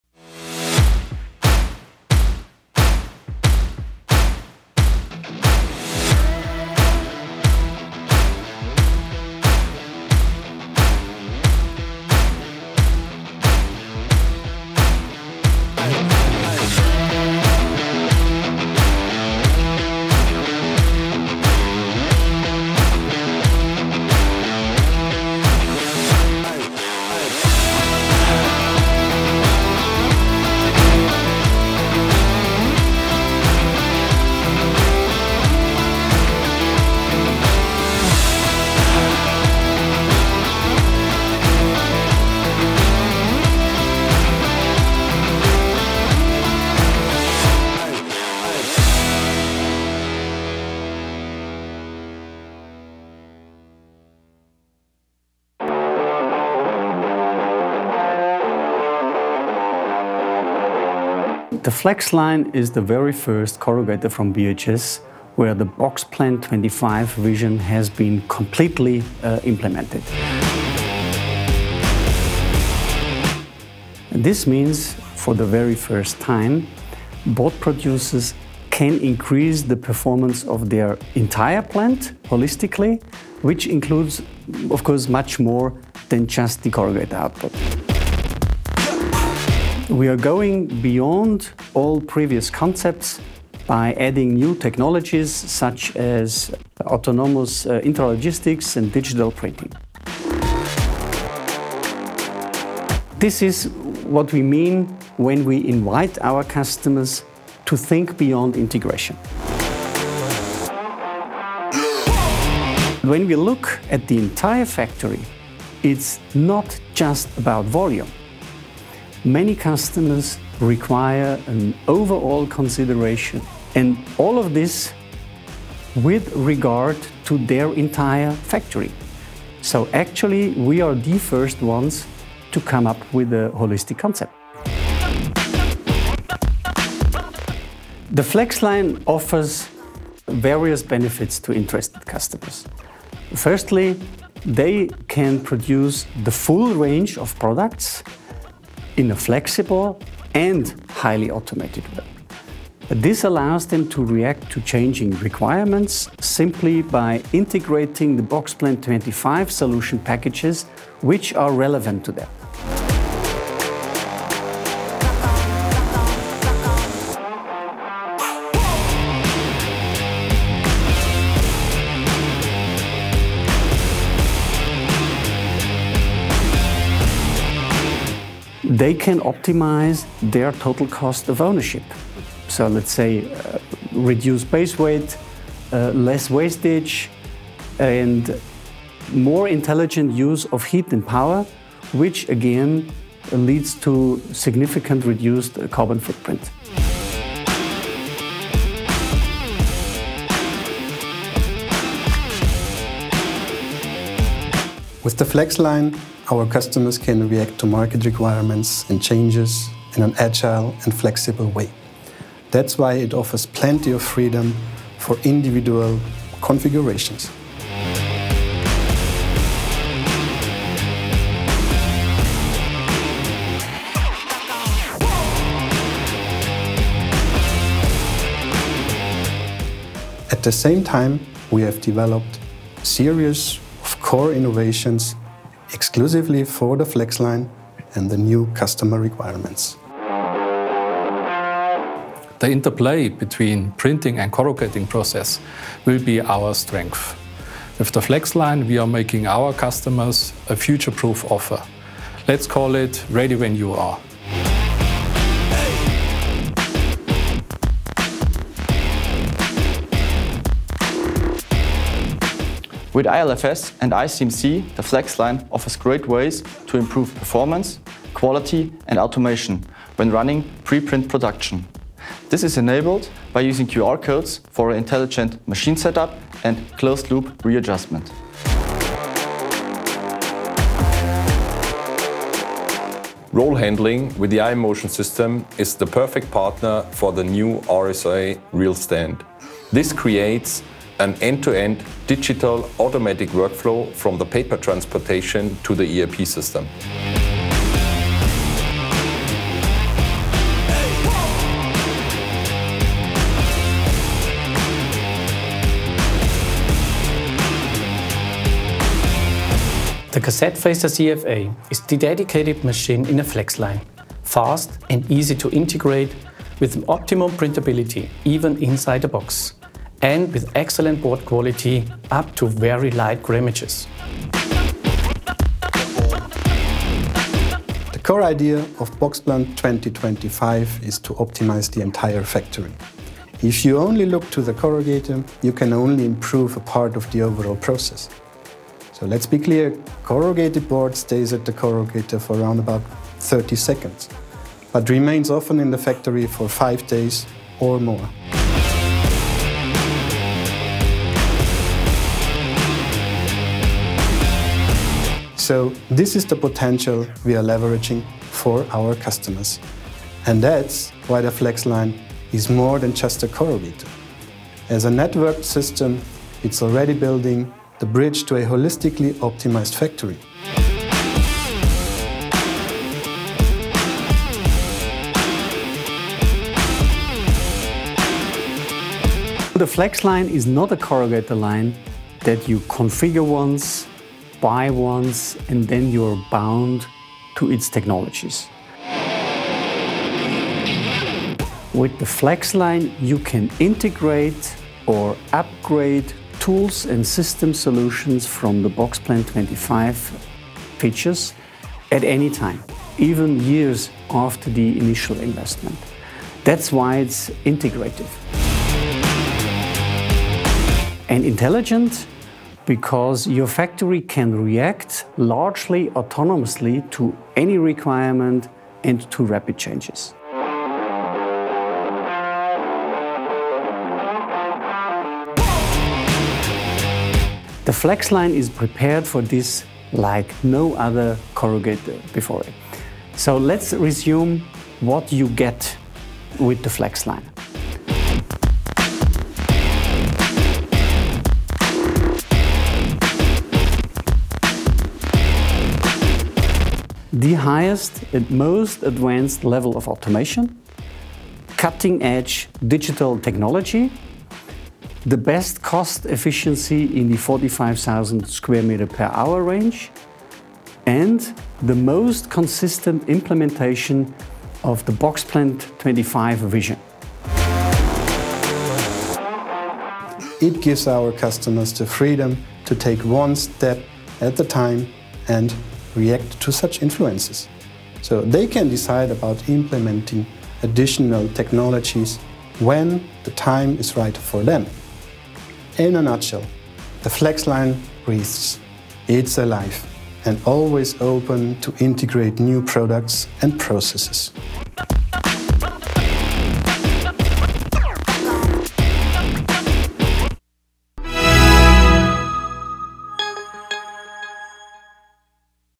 The FLEX Line from BHS Corrugated – More than just a Corrugator Line
Discover the future of the corrugated box plant with the FLEX Line from BHS Corrugated!
Our exclusive launch video provides profound insights into this highly automated and intelligent corrugator that redefines flexibility and integration. Learn more about the standards it sets in flexibility, digitalization, and automation, as well as its seamless integration of BHS Jetliner digital printing systems and autonomously controlled iMotion intralogistics.

Dive in and look forward to the future of the corrugated box plant! 🚀

Box Plant 2025 – think beyond integration.

0:00 Intro
1:00 THE FLEXLINE